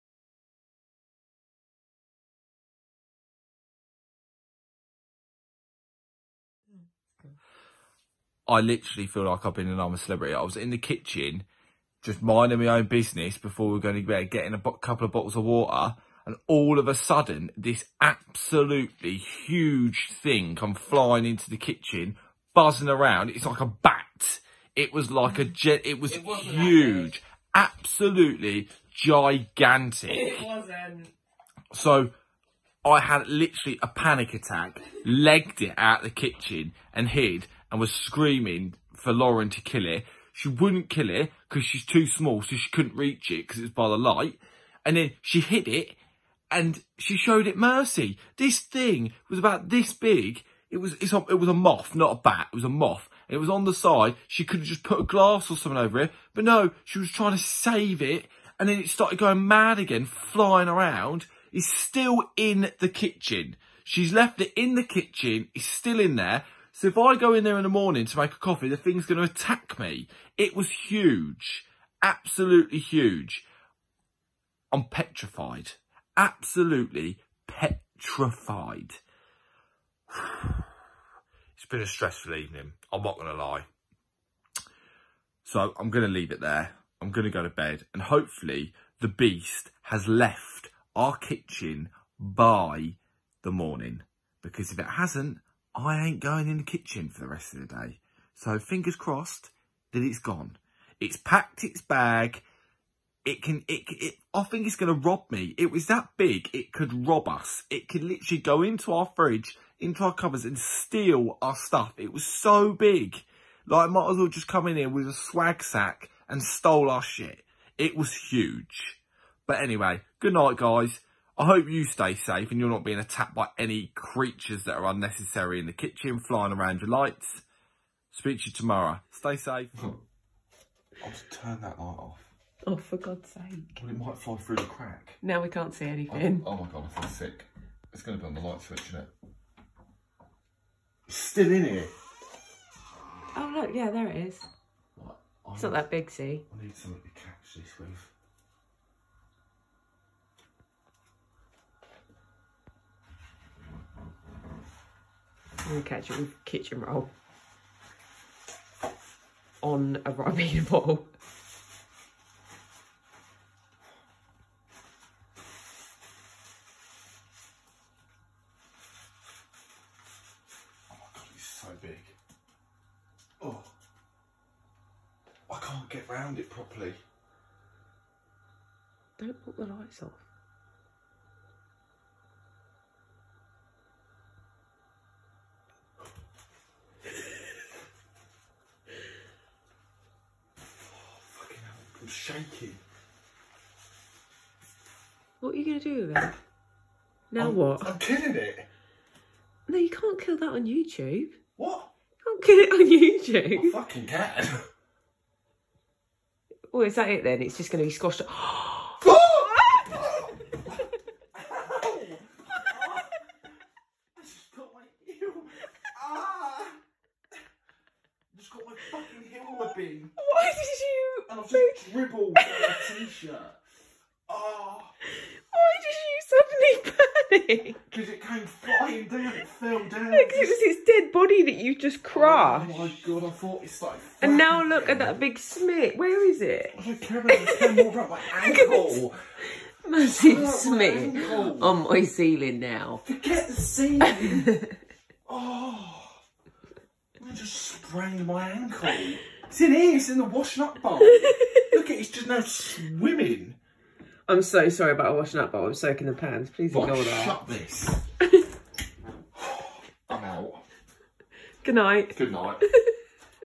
VLOG 3: MIDNIGHT ANTICS | FUNNIEST VIDEO | ATTACK OF THE MOTH!! Watch to the end.
I had to get this uploaded for you straight away, randomly filmed it last night. It has had Lauren & I in stitches all day! I hope it makes you all laugh too... at my expense.

Thank you SO MUCH to all those who are already following my new YouTube journey.

Stay Safe.